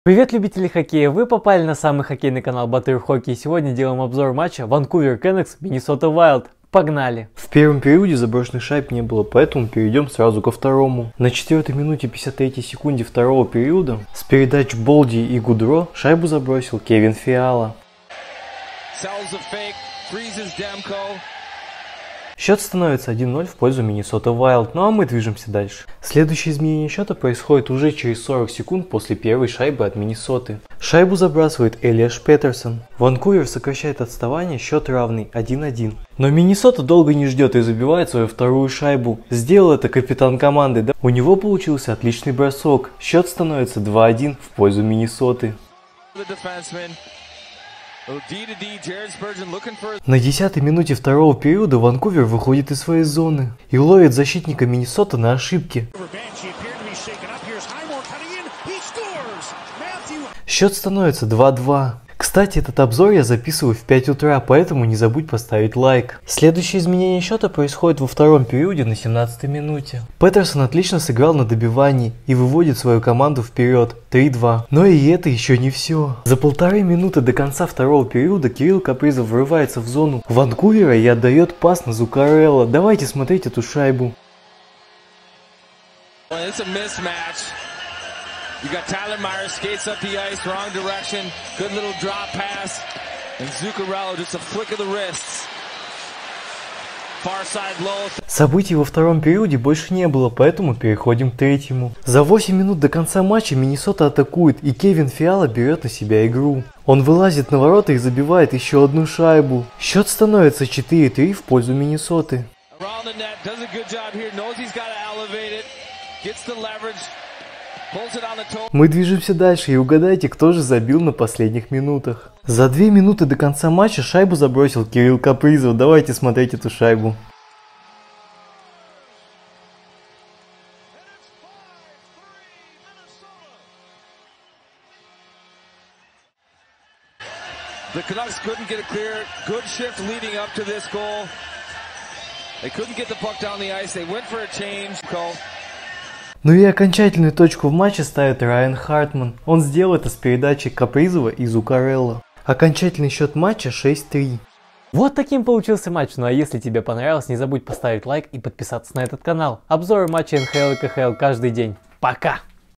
Миннесота Вайлд - Ванкувер Кэнакс Обзор матча НХЛ(22.04.2022)
Миннесота Вайлд - Ванкувер Кэнакс Обзор матча НХЛ(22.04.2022) лучшие моменты и голы Кирилла Капризова и его команды!